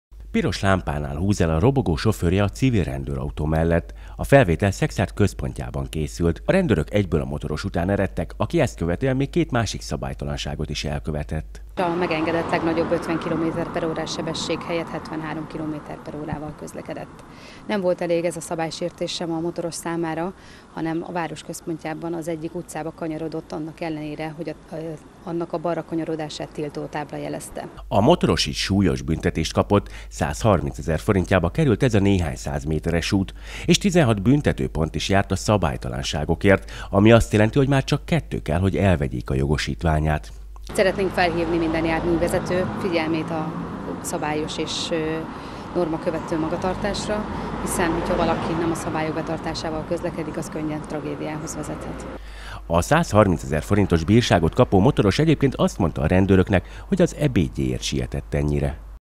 130 ezer forint bírság néhány száz méteren - 3 súlyos szabálysértést is elkövetett a robogós
130 ezer forint bírságot kapott egy robogós Szekszárdon, mert néhány száz méteren több szabálysértést is elkövetett. Az egészet videóra vették a rendőrök. A motoros emellett 16 büntetőpontot is bezsebelt, ami azt jelenti, hogy mostantól nagyon könnyen elveszítheti a jogosítványát.